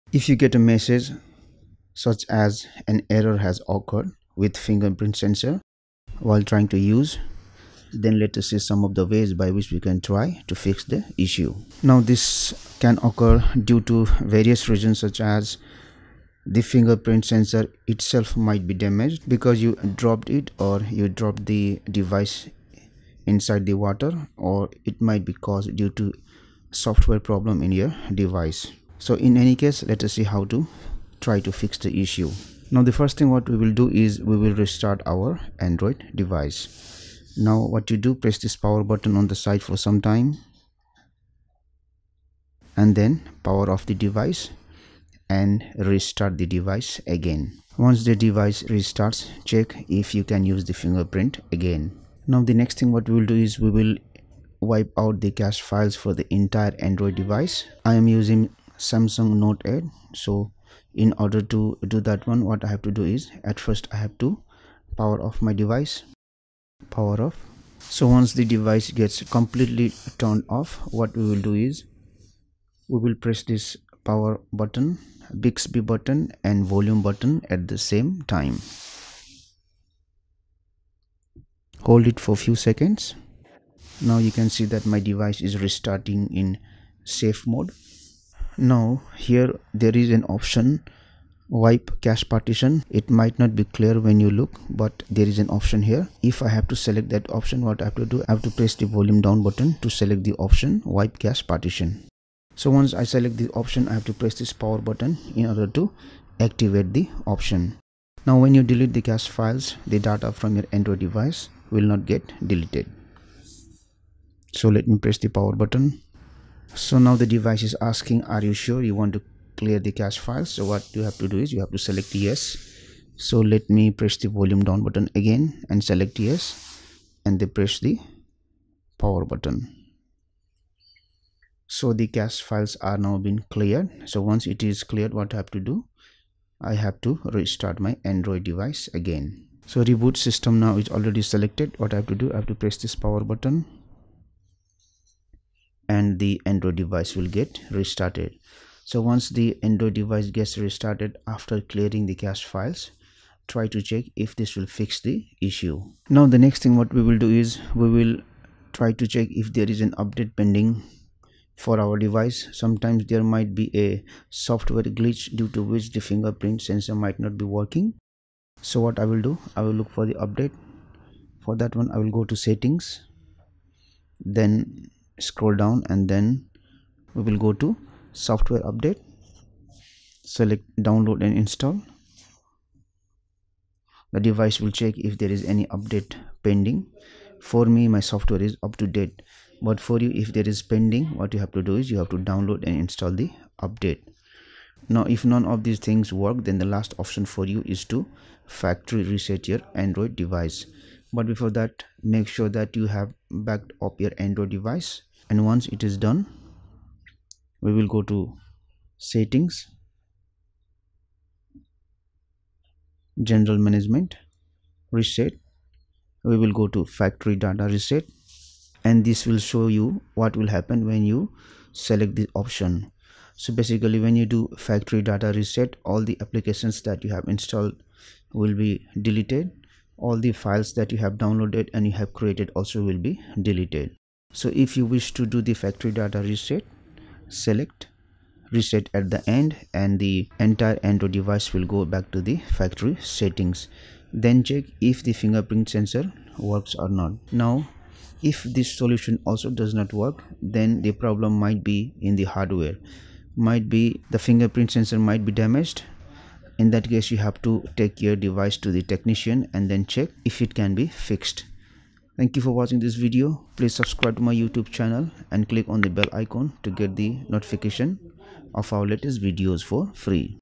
How to fix An error has occurred with finger print sensor. Restart your device
In this video today we will see what to do if you get an error message such as An error has occurred with finger print sensor and should restart your device.

If you get a message such as an error has occurred with fingerprint sensor while trying to use then let us see some of the ways by which we can try to fix the issue. Now this can occur due to various regions such as the fingerprint sensor itself might be damaged because you dropped it or you dropped the device inside the water. Or it might be caused due to software problem in your device. So in any case let us see how to try to fix the issue. Now the first thing what we will do is we will restart our android device. Now what you do press this power button on the side for some time

and then power off the device and restart the device again. Once the device restarts check if you can use the fingerprint again. Now the next thing what we will do is we will wipe out the cache files for the entire android device. I am using Samsung note 8. So in order to do that one what i have to do is at first i have to power off my device power. Off. So once the device gets completely turned off what we will do is we will press this power button, bixby button and volume up button at the same time.

Hold it for few seconds. Now you can see that my device is restarting in safe mode. Now here there is an option "wipe cache partition". It might not be clear when you look but there is an option here. If i have to select that option what i have to do, i have to press the volume down button to select the option wipe cache partition. So once i select the option i have to press this power button in order to activate the option. Now when you delete the cache files the data from your android device will not get deleted. So let me press the power button. So now the device is asking, Are you sure you want to clear the cache file. So what you have to do is you have to select "Yes". So let me press the volume down button again and select yes and they press the power button.

So the cache files are now been cleared. So once it is cleared what I have to do I have to restart my android device again. So reboot system now is already selected. What I have to do I have to press this power button

and the android device will get restarted. So once the android device gets restarted after clearing the cache files try to check if this will fix the issue. Now the next thing what we will do is we will try to check if there is an update pending for our device. Sometimes there might be a software glitch due to which the fingerprint sensor might not be working. So what I will do, I will look for the update. For that one I will go to "settings". Then scroll down and then we will go to "software update". Select "download and install". The device will check if there is any update pending. For me my software is up to date but for you if there is pending what you have to do is you have to download and install the update. Now if none of these things work then the last option for you is to factory reset your android device. But before that make sure that you have backed up your android device and once it is done we will go to

"settings" - "General Management" - "Reset". We will go to "Factory data reset" and this will show you what will happen when you select the option. So basically when you do factory data reset all the applications that you have installed will be deleted. All the files that you have downloaded and you have created also will be deleted. So if you wish to do the factory data reset, select "Reset" at the end and the entire android device will go back to the factory settings. Then check if the fingerprint sensor works or not. Now if this solution also does not work then the problem might be in the hardware. Might be the fingerprint sensor might be damaged. In that case you have to take your device to the technician and then check if it can be fixed.